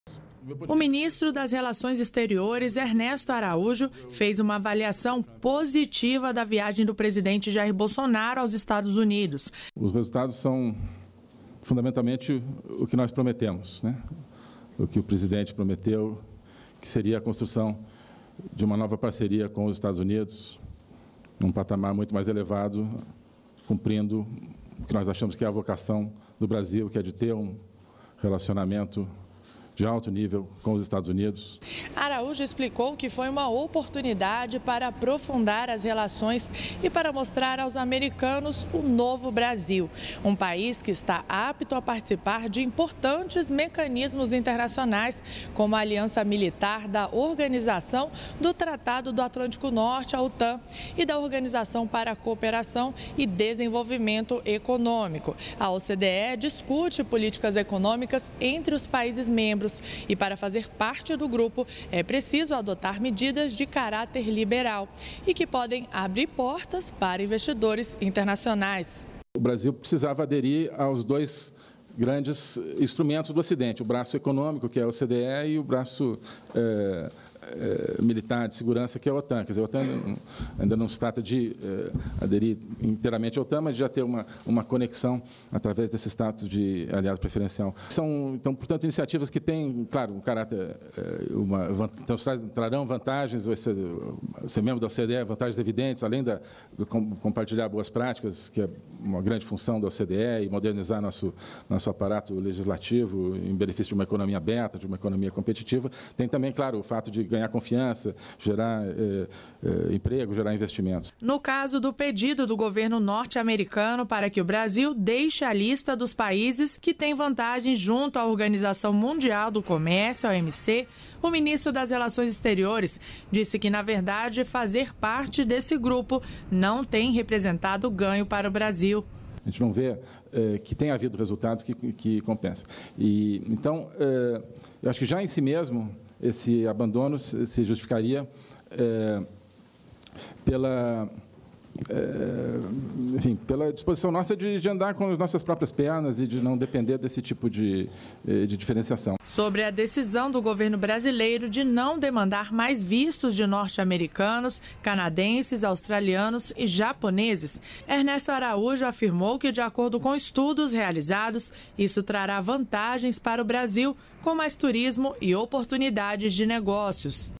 Ministro das Relações Exteriores faz Balanço da visita aos Estados Unidos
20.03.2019 -  Os dois países concordaram em aprofundar a parceria no combate ao terrorismo, ao tráfico de armas e drogas, aos crimes cibernéticos e à lavagem de dinheiro por meio do Fórum Permanente de Segurança Brasil-EUA, e saudaram a assinatura de dois instrumentos para melhorar a segurança de fronteira. O Presidente Bolsonaro anunciou a intenção de isentar cidadãos dos EUA de vistos de turista, e os Presidentes concordaram em dar os passos necessários para permitir a participação do Brasil no Programa de Viajantes Confiáveis “Global Entry” do Departamento de Segurança Interior.